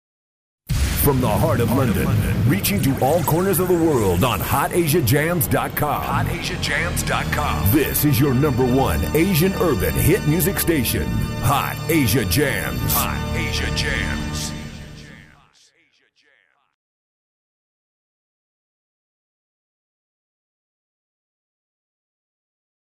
Hot Asia Jamz Radio Trailer July 2014
The newest most anticipated urban asian dance radio station in London - Hot Asia Jamz. Coming to a frequency near you.